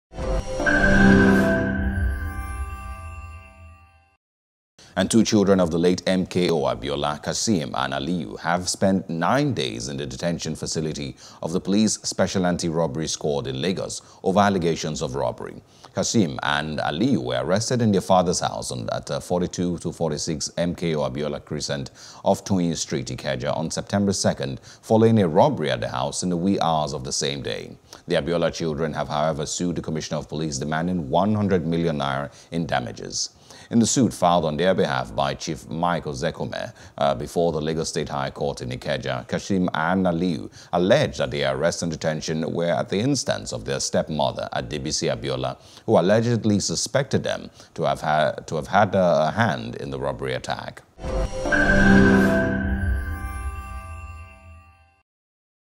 MKO Abiola’s Sons Spend Nine Days in Detention (NEWS | NIGERIA)
Two children of the late MKO Abiola, Kassim and Aliyu, have spent nine days in the detention facility of the police Special Anti-Robbery Squad in Lagos over allegation of robbery.